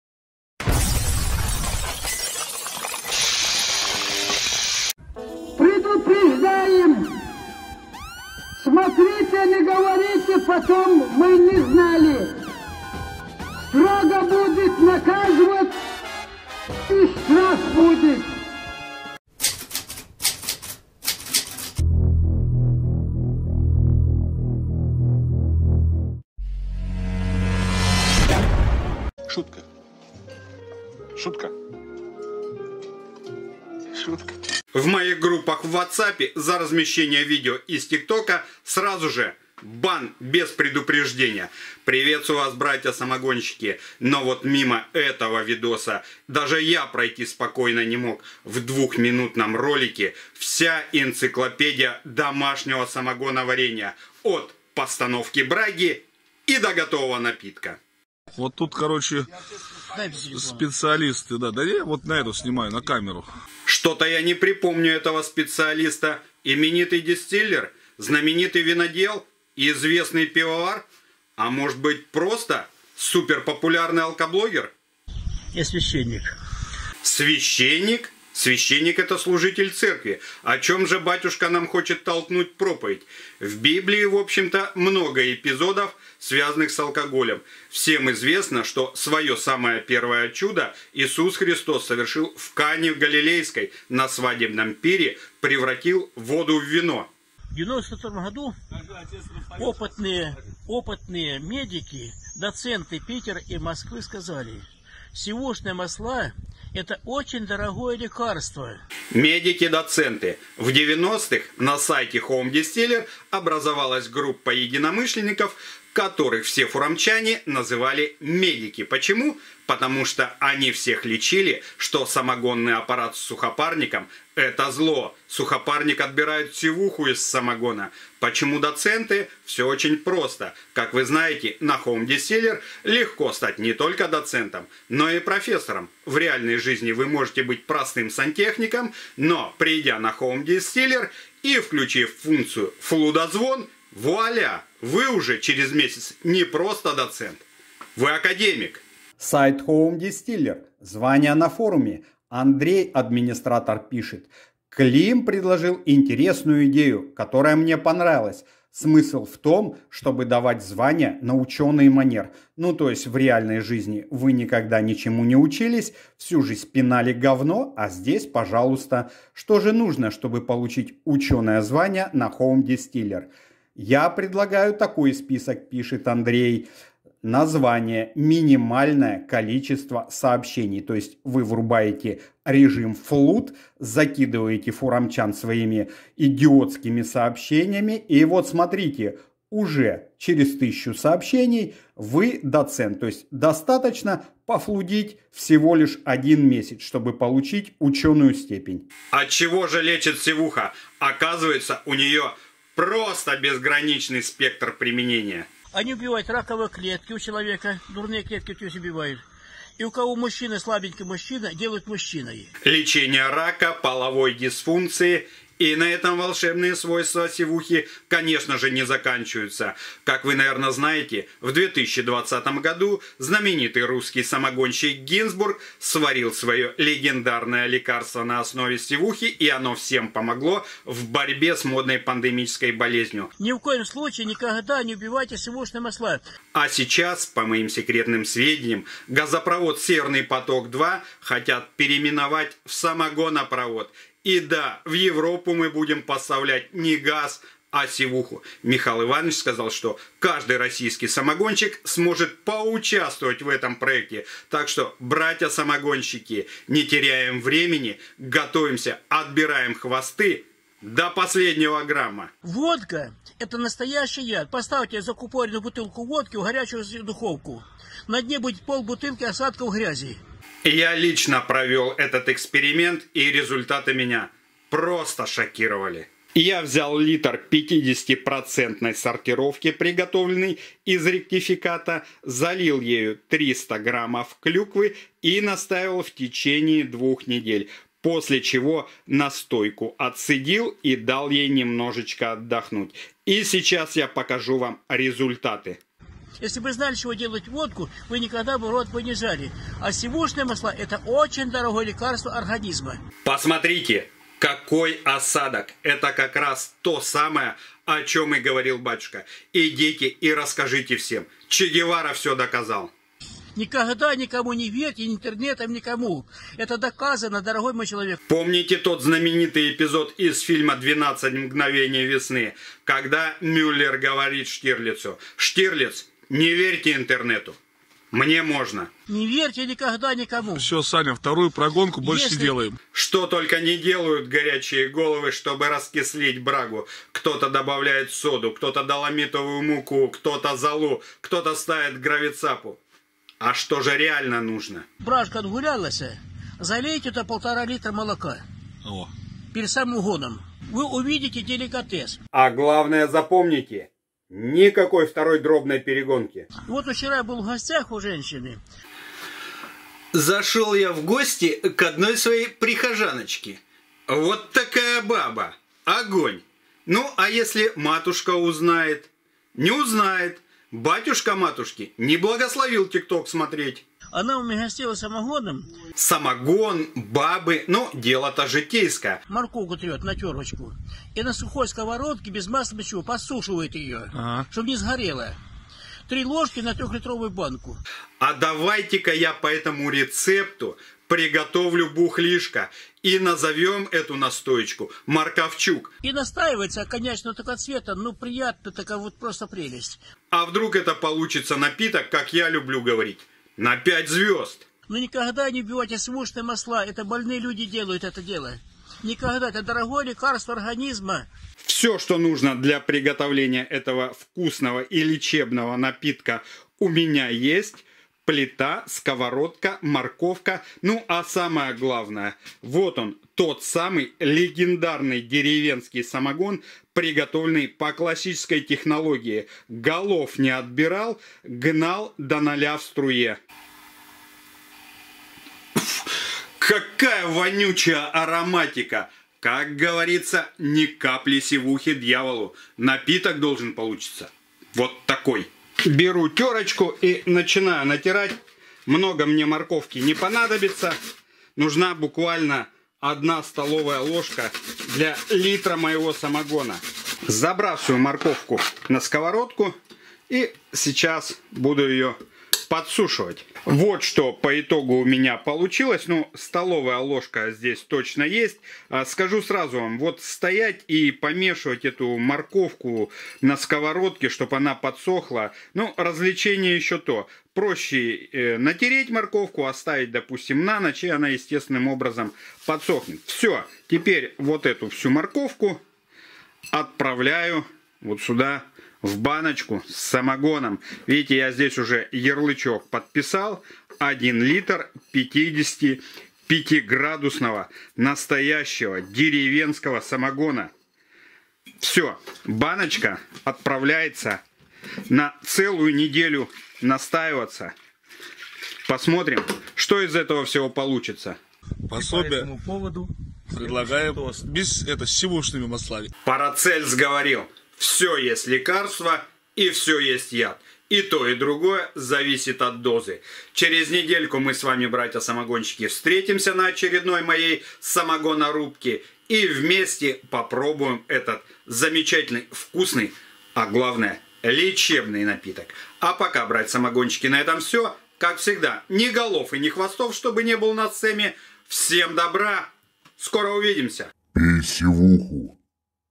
Морковная настойка на самогоне. Рецепт от православного батюшки из Tik Toka
Уникальным рецептом морковной настойки на самогоне поделился со мной один православный батюшка, которого я нашел в Tik Toke.